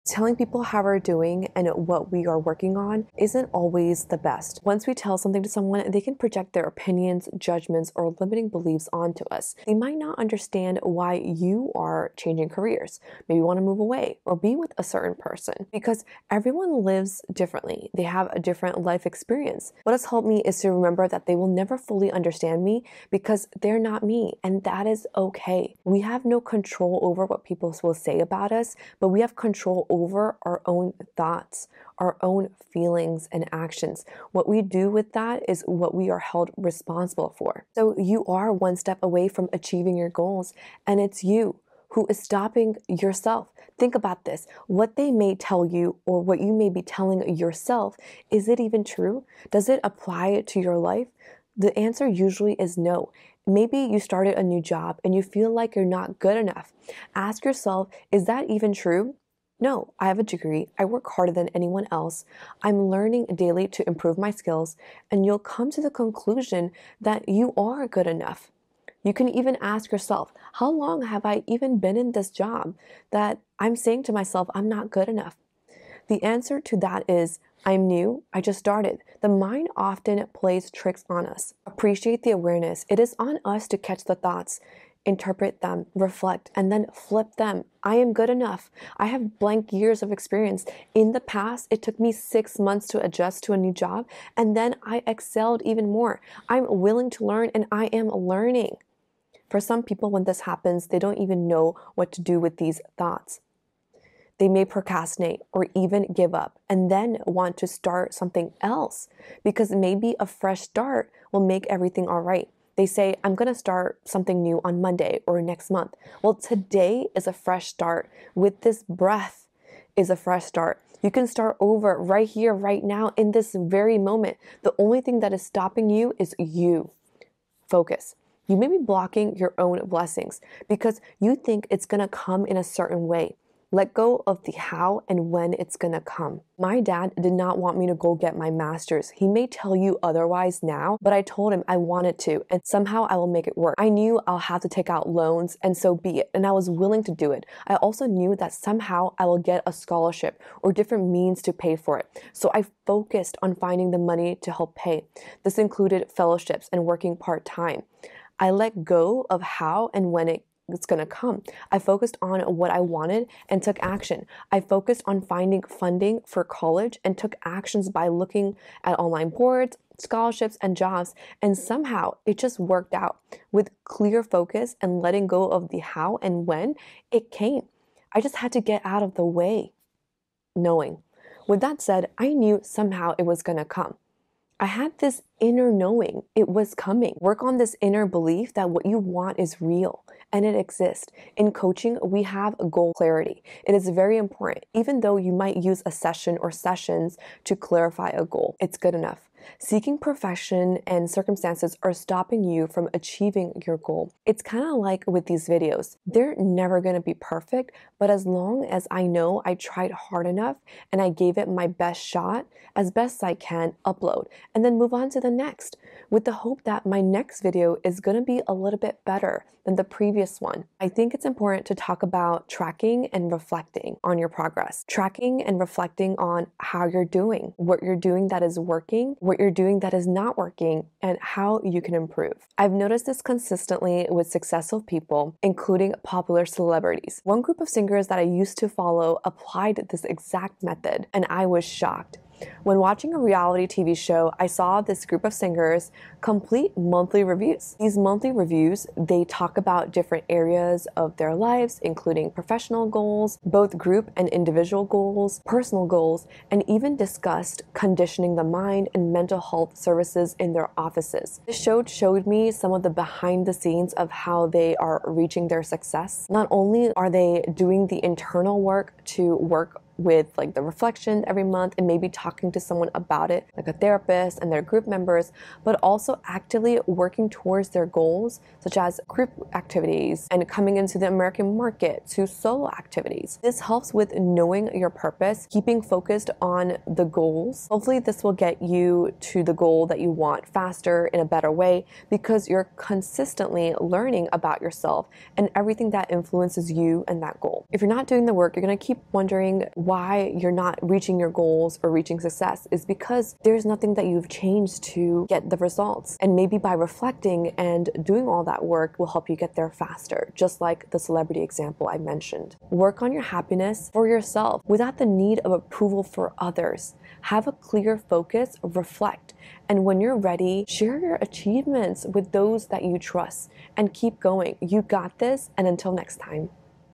You're ONE Step Away from Achieving Your Goals before 2025 | Unlock Your FULL POTENTIAL NOW
Are you ready to achieve your goals before 2025? You're just one step away from making it happen. Stop telling people about your dreams and start taking action. Your inner belief is the key to unlocking your potential. Sharing personal information with others can lead to them projecting their opinions, judgments, or limiting beliefs onto us. People may not understand our choices because everyone's life experiences are different. It's important to remember that others may never fully understand us, and that's okay. Maintaining boundaries, working in silence, and focusing on our own thoughts and actions can be empowering. We can't control what others say about us, but we can control our own feelings, thoughts, and actions. It's essential to challenge thoughts and beliefs that may not be true or relevant to our lives. By questioning these thoughts, we can realize our worth and capabilities. Remember, the mind can sometimes deceive us. I discuss in the video a simple question you can ask when this happens.

Embrace self-awareness and challenge negative thoughts by affirming your worth and experience. Remember past successes and your willingness to learn and grow. Don't let self-doubt lead to procrastination or giving up. Every moment is a fresh start to take action and overcome obstacles. Start now and don't let self-limiting beliefs hold you back.

Let go of the need to control the 'how' and 'when'. Don't limit your blessings by expecting them to come in a specific way. Focus on your goals, take action, and let go of the how and when. By staying focused and open to possibilities, opportunities will present themselves. Just trust the process and allow things to fall into place.

Remember, it's good enough. You are capable and deserving of success. Believe in your abilities and keep pushing forward. Don't let perfectionism hold you back from reaching your goals. Focus on doing your best and making progress, rather than seeking perfection. Track and reflect on your progress to identify what's working, what's not, and how you can improve.

Achieving your goals faster and more effectively involves staying focused, reflecting, adjusting, and constantly learning about yourself. Regular reviews, monthly reflections and or just reflections, along with tracking progress, are key to success.

You have the power to make your dreams a reality. Take that one step today and watch as you reach your goals before 2025. You've got this!